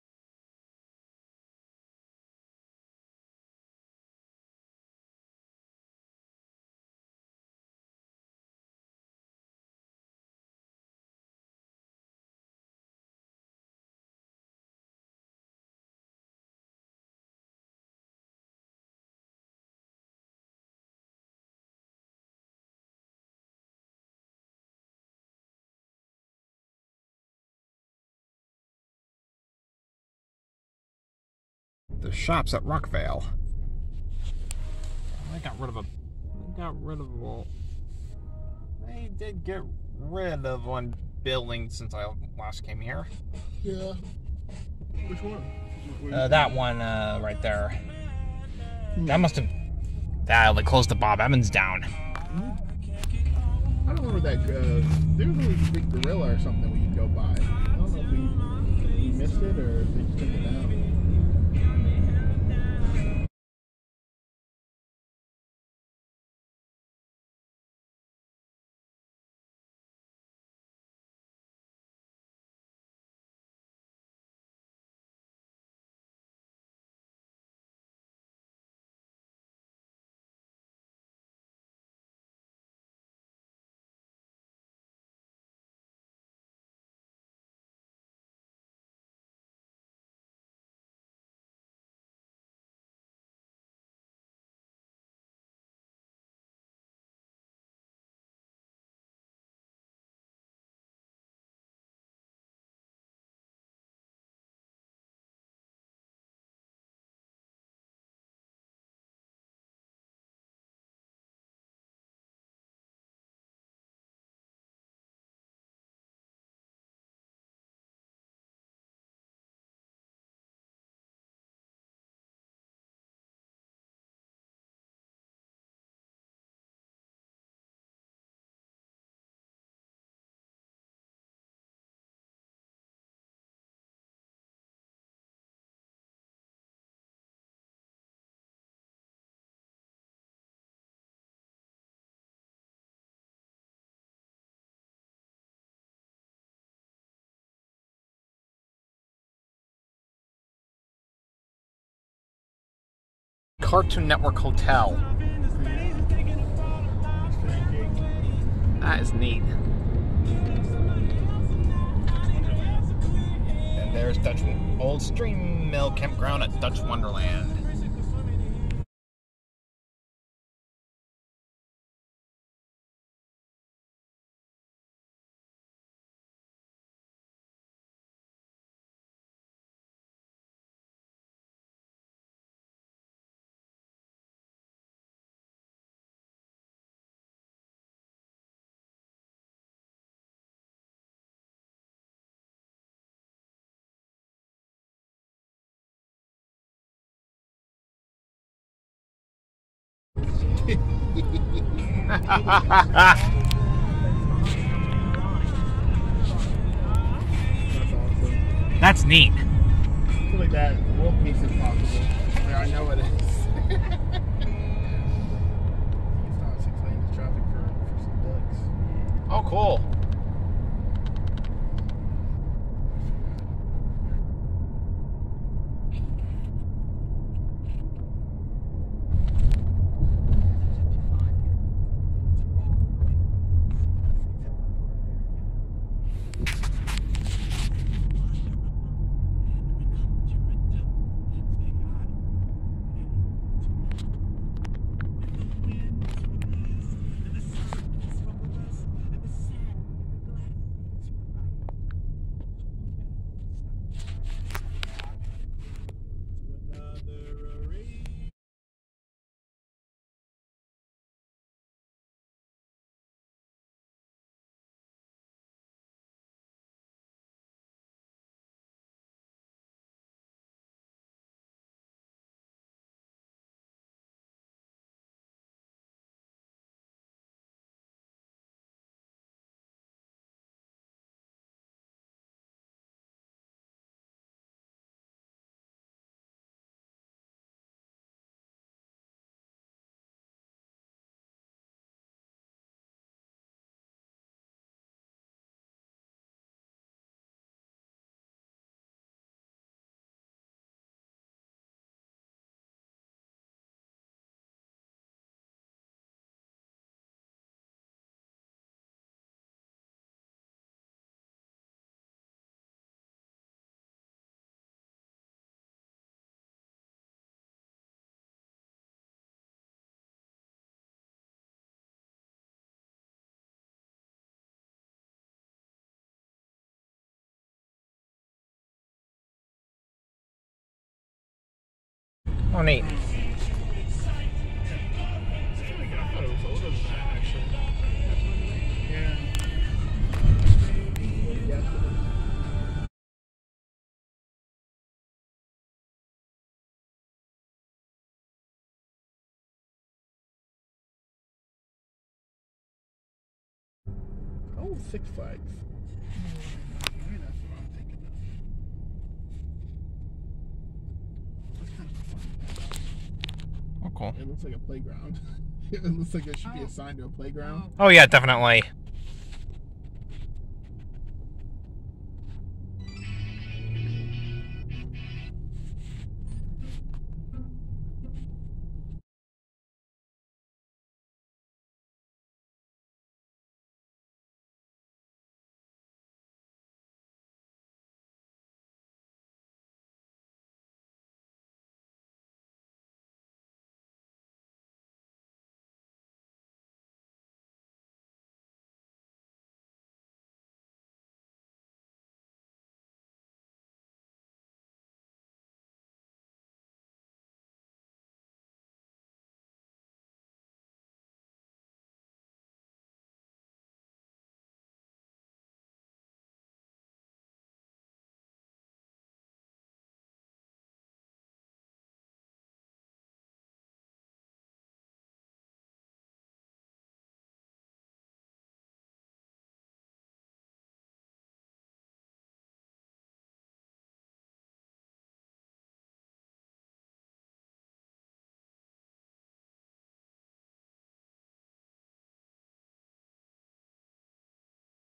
Random Video: Riding through the Roads and city of Lancaster, Pennsylvania (5/21/2023)
I decided to capture parts of me on the roads in the back seat of the car and get some video shots of Lancaster while coming back home from our 3 day trip in Wildwood, NJ So, enjoy some of the shots, but please note there will be audio muting due to the radio being on.